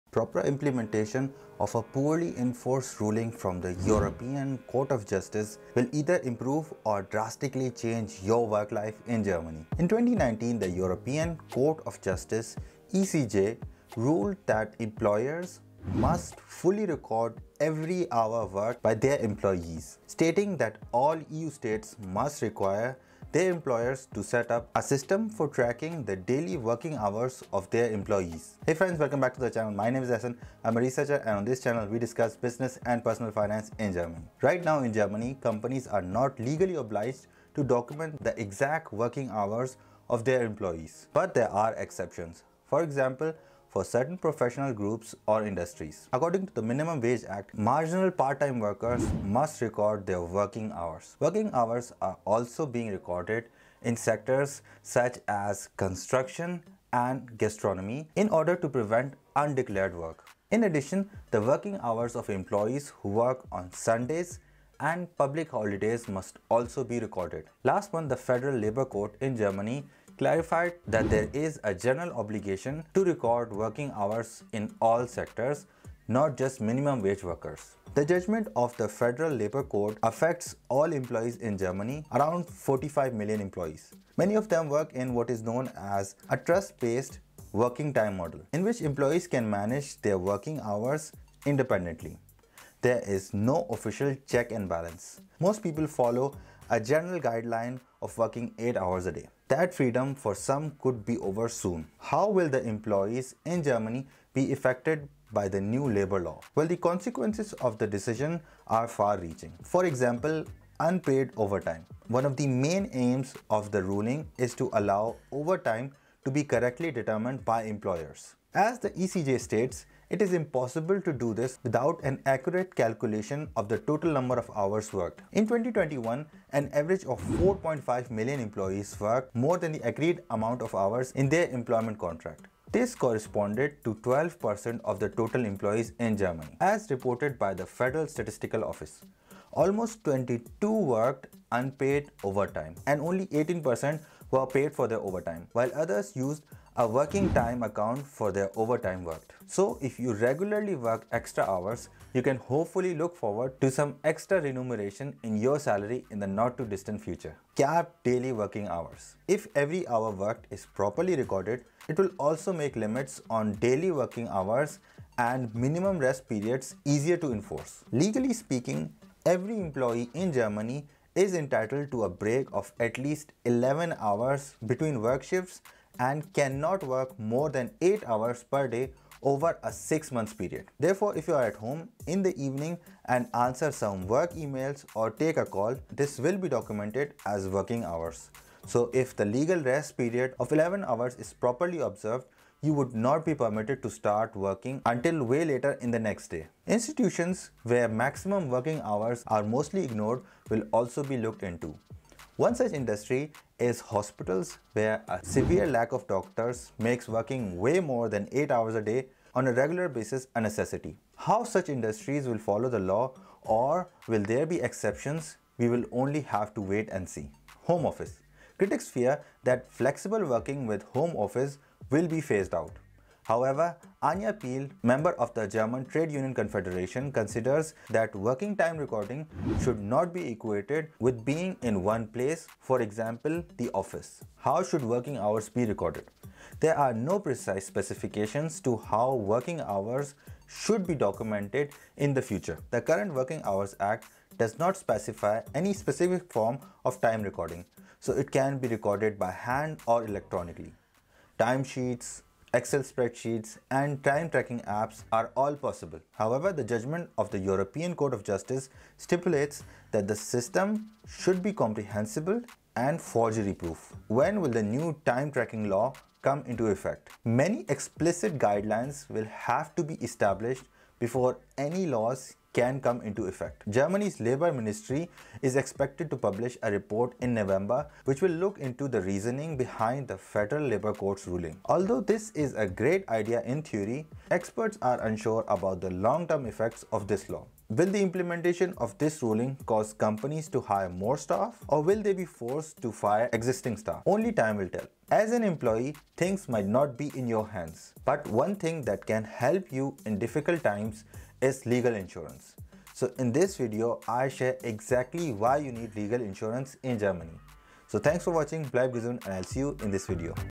NEW LAW that will Improve or Change your WORK LIFE in Germany
In this video I discuss a Labor Law that will either Improve or Change your work life in Germany

Equipment Used to Make the video//
🎙️ Mic for pc *
💡 Lighting setup*

In 2019 the European Court of Justice (ECJ) ruled that employers must fully record every hour worked by their employees. Stating that all EU states must require their employers to set up a system for tracking the daily working hours of their employees.
How will employees in Germany be affected by the new labour laws?
How should working hours be recorded?
When will the new time Tracking law come into effect?